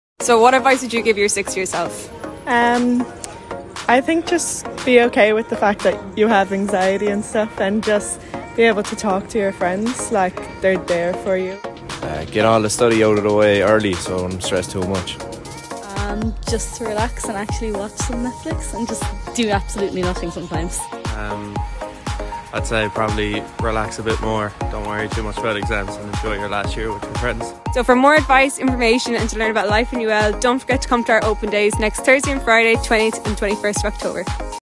"What advice would you give your sixth year self?"
Ahead of UL Open Days, where we'll welcome thousands of senior cycle students to our campus, we asked current UL students what advice they'd give their sixth year self, looking back now...